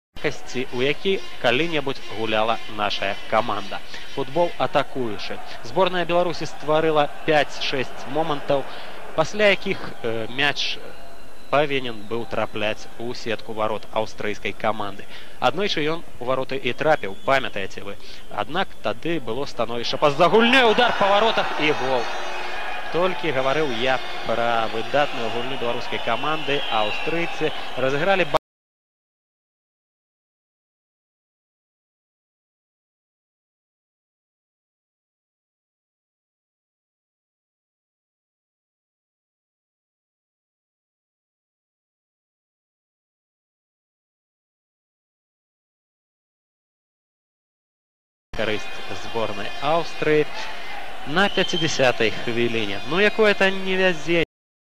Belarus vs Austria (World Cup 1998 Qualifier)
World Cup 1998 Qualifier (UEFA, Group 4)
10 September 1997, Dinamo, Minsk (7,000)
Referee: Serge Muhmenthaler (Switzerland)

                           Belarus  0-1  Austria
                                                  Pfeifenberger 50'